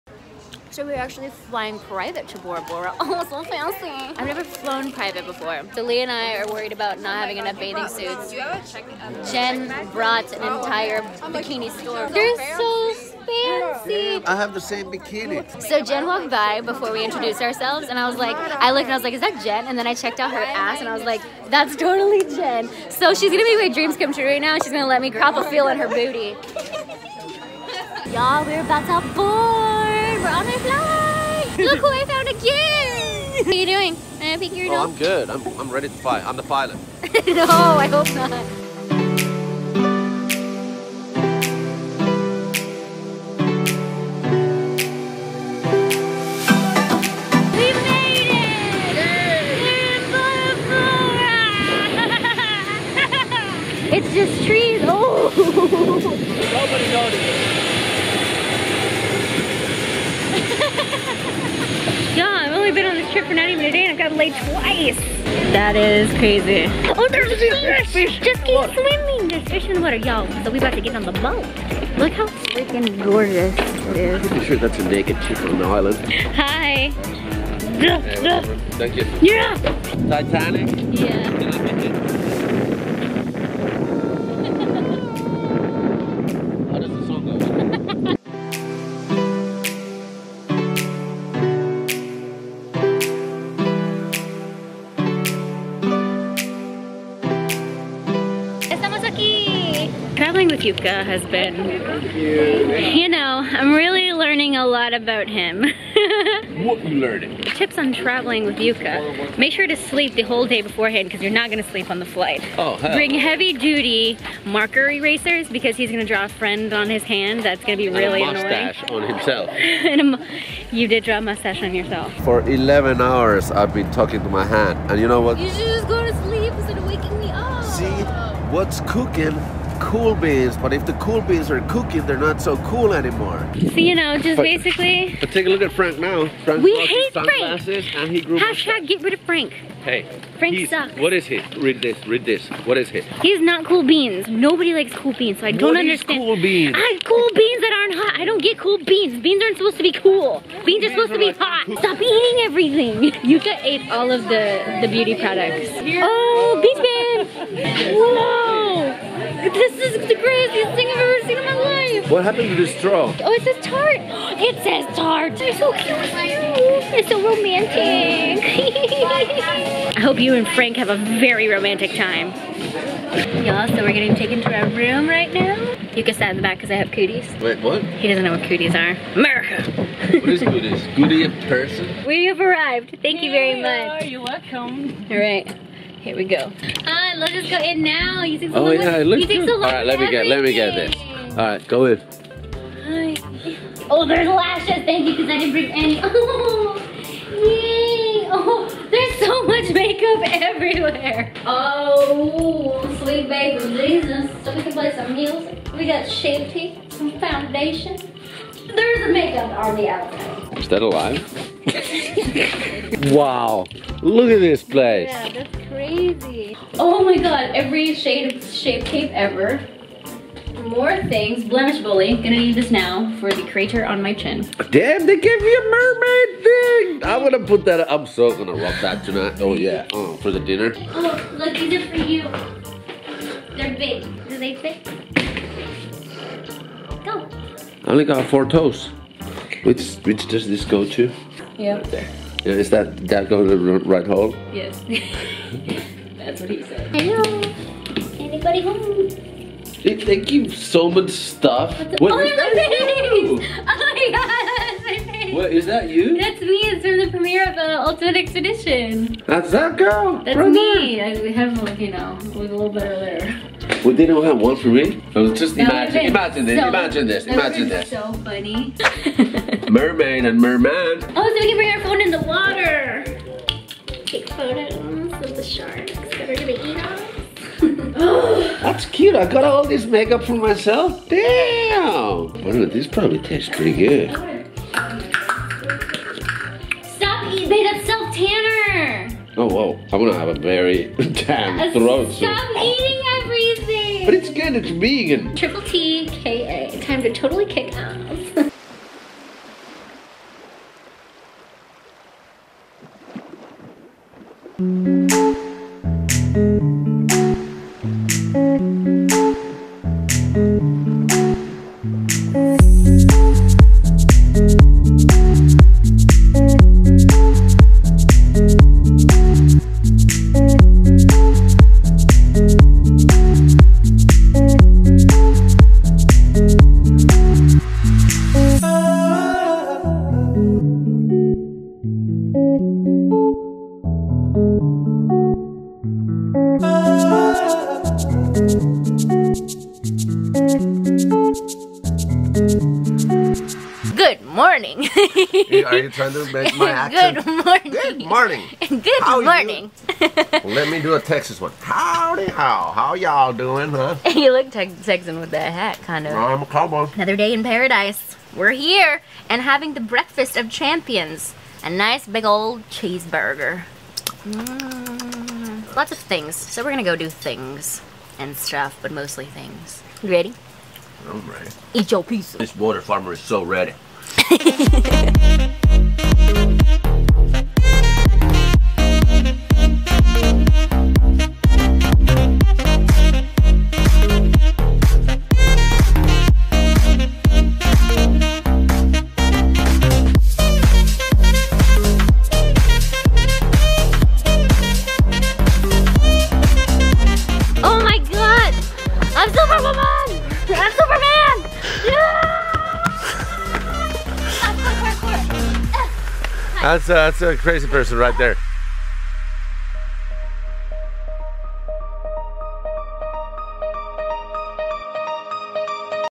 Jet skis and Jukka in Bora Bora!
Had a blastyyyyy blast in Bora Bora with Jukka! Hope y’all enjoy our shenanigans:)

Thanks to @tartecosmetics for treating us to an unforgettable experience!